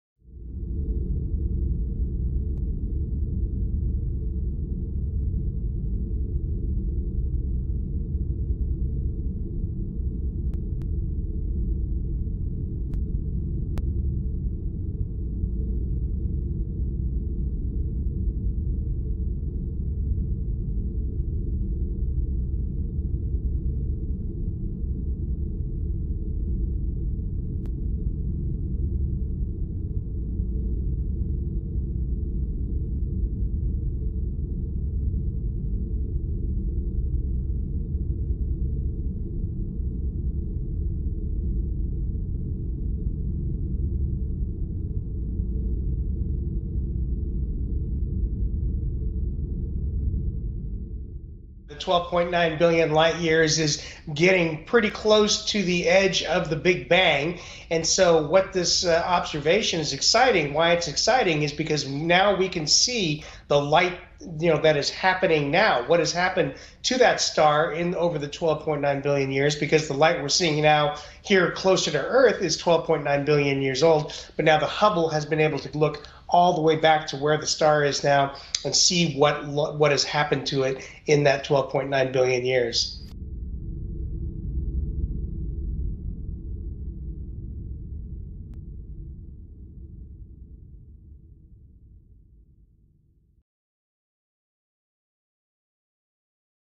Le télescope Hubble découvre l'étoile la plus lointaine jamais observée
Le télescope spatial Hubble a établi un nouveau record pour la prise de vue d'une étoile unique et lointaine.

La lumière de l'objet a mis 12,9 milliards d'années pour atteindre la Terre. Elle a été baptisée "Earende", ce qui signifie "étoile du matin" en vieil anglais.

Les scientifiques estiment que "Earende" a une masse au moins 50 fois supérieure à celle du Soleil et est des millions de fois plus brillante, ce qui la place parmi les étoiles les plus massives connues.